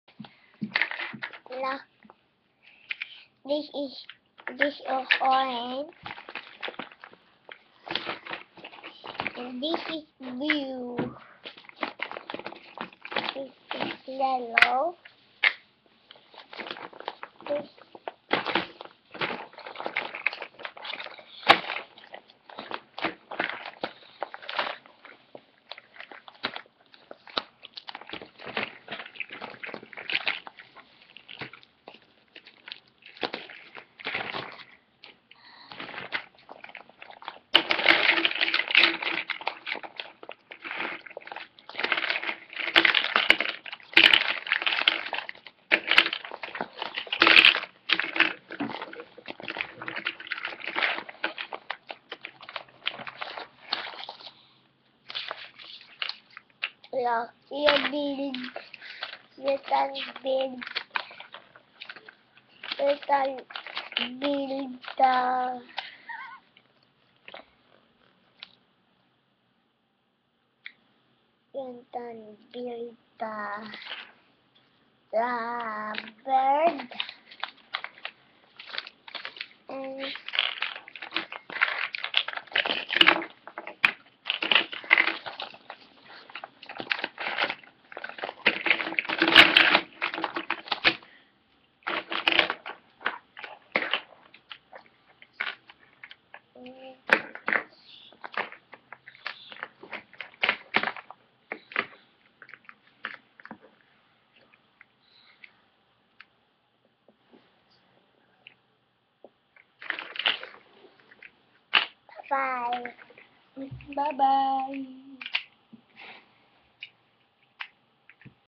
BLOCKS Play and Learn
Today, we have a new toy BLOCKS.  It's like LEGO but bigger.